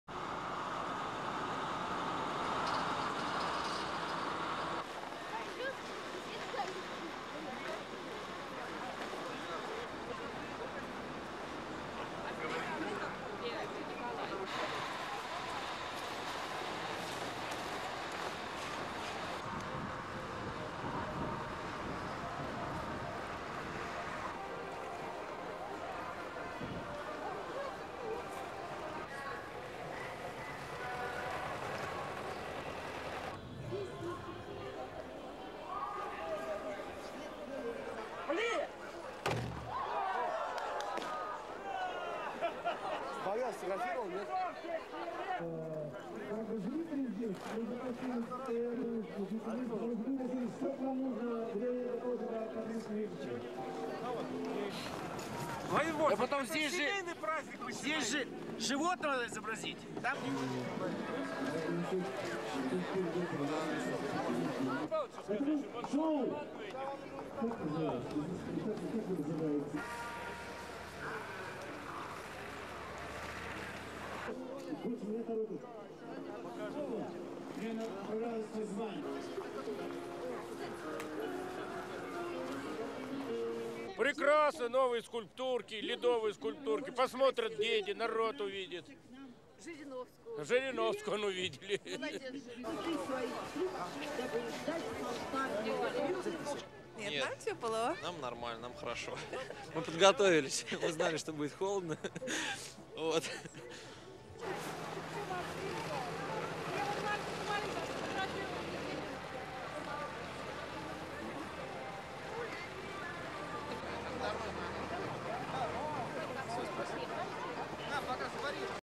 Annual festival of ice sculptures
1. Compound of Economic Exhibition Center in Moscow
2. People near ice sculptures
3. Huge ice ball
4. Ice columns with children playing
5. Girl walking along ice wall
6. Girl touching ice wall
7. Woman in fur coat
8. Ice replica of Moscow's Bolshoi Theatre
9. Ice cannon firing
10. People watching sculptures
11. Russian politician Vladimir Zhirinovsky taking part in cutting a sculpture
12. Sculptor's hands
13. Zhirinovsky cutting a bear out of ice
14. Ice figure of woman
15. Girl
16. Ice replica of television tower
17. Ice sculptures
18. SOUNDBITE (Russian) Viktor Petrovich, pensioner
"Beautiful ice sculptures. Children, all people can seem them. They can also see Zhirinovsky here."
19. People looking at sculptures
20. SOUNDBITE (Russian) Alexander and Irina
"It is nice and warm here. Not cold, we have prepared for the weather."
21. Ice wall
22. Girl touching ice sculpture
23. Hand on ice

STORYLINE:

The city of Moscow hosted on Sunday the annual festival of sculptures, carved from ice cubes by more than twenty artists from all over Russia.

The unveiling of the festival, which is expected to last several days at the All-Russian Exhibition Complex, attracted several hundred people who braved minus 10 degree Centigrade frost and high humidity.

Russia's flamboyant politician Vladimir Zhirinovsky was seen among the artists, meticulously carving an ice bear, apparently reflecting his new leaning towards the ruling Unity party whose mascot is the bear.